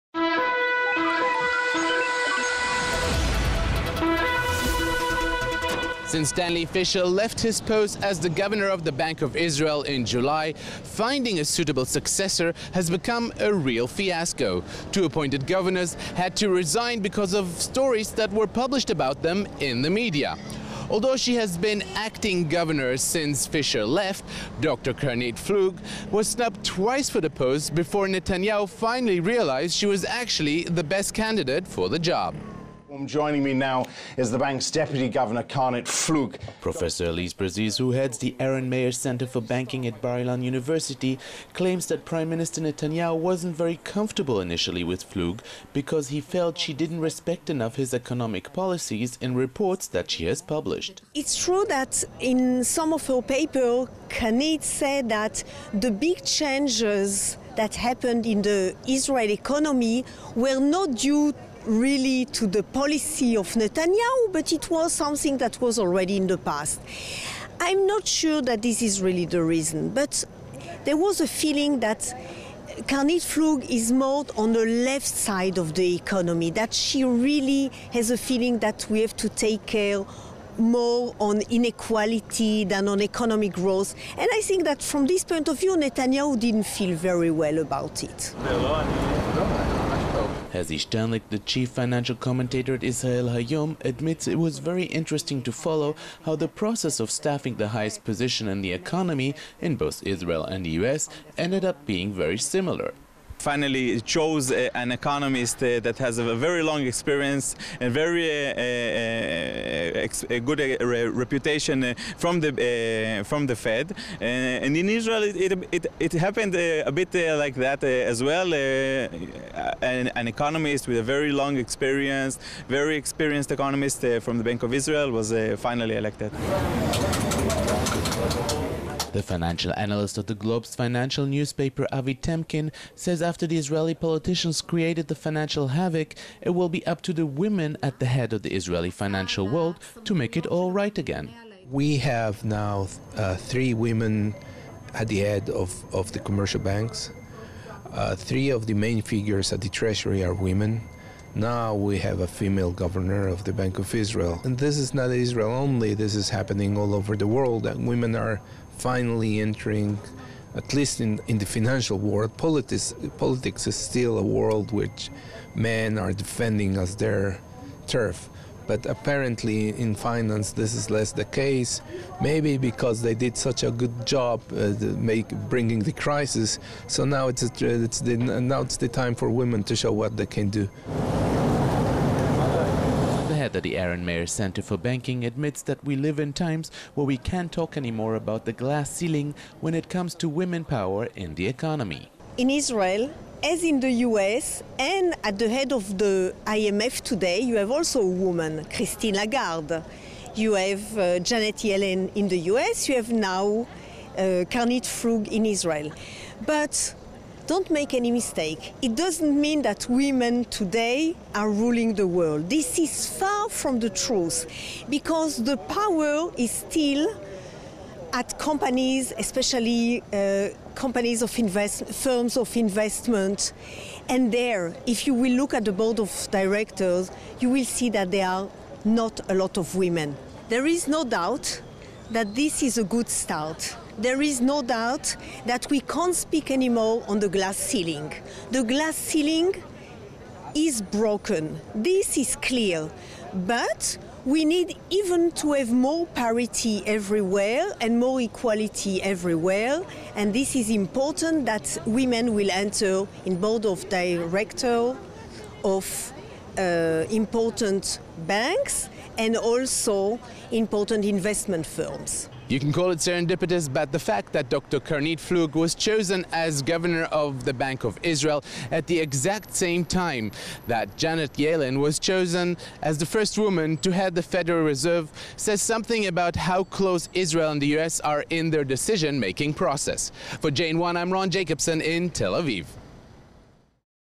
Introducing Israel's new chief banker: Karnit Flug becomes first female governor of Bank of Israel
Since Stanly Fisher left his post as the governor of the Bank of Israel in July, finding a suitable successor has become a real fiasco. Two appointed governors had to resign because of stories that were published about them in the media. Although she has been acting governor since Fisher left, Dr. Karnit Flug, was snubbed twice for the post before Netanyahu finally realized she was actually the best candidate for the job.